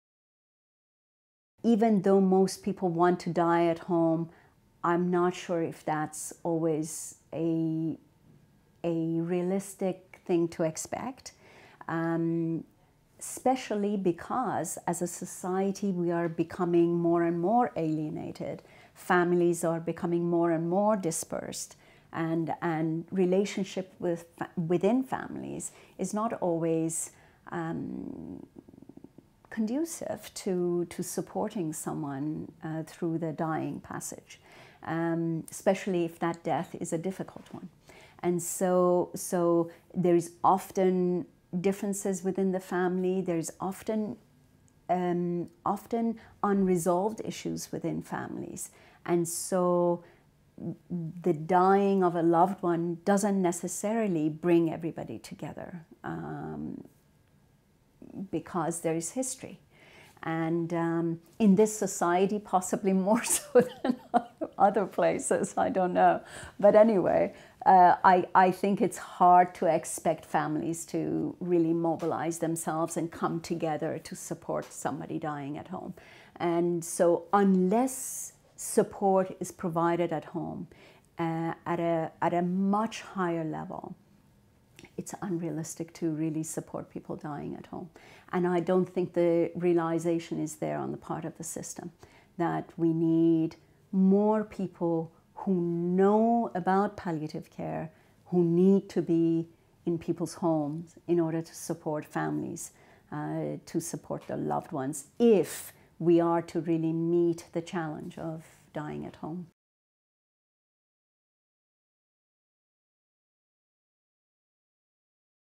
Simin: The challenge of helping someone to die at home
In this video by Canadian Virtual Hospice, Simin discusses what is needed to make dying at home a practical choice.